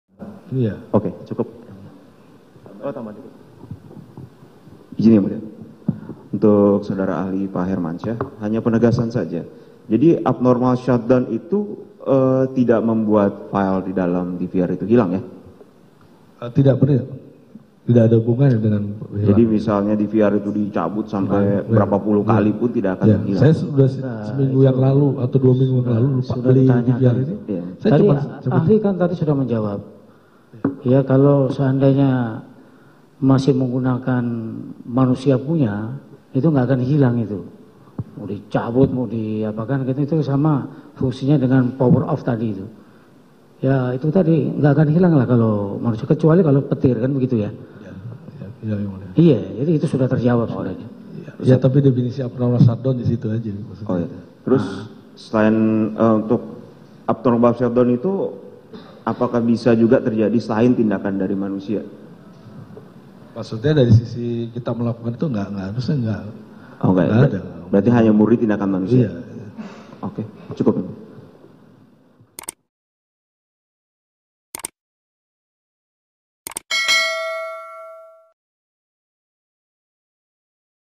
Sidang Arif Rachman Arifin, Saksi Ahli Sebut Abnormal Shutdown Tak Buat File di DVR CCTV Hilang
Saksi ahli meringankan pada sidang Arif Rachman Arifin, Hermansyah mengatakan abnormal shutdown tidak berpengaruh dalam perusakan file dalam DVR CCTV di dalam kasus perintangan penyidikan di kasus pembunuhan Brigadir J.

Hermansyah menyebut, file di dalam DVR CCTV hanya akan hilang jika hard disk di dalamnya rusak total. Hal tersebut juga ia simulasikan saat persidangan, Jumat (20/1/2023).

Hermansyah juga memastikan jika abnormal shutdown itu tindakan murni dari manusia.

Video Jurnalis: Syalutan Ilham
Penulis Naskah: Syalutan Ilham
Video Editor: Syalutan Ilham
Produser: Okky Mahdi Yasser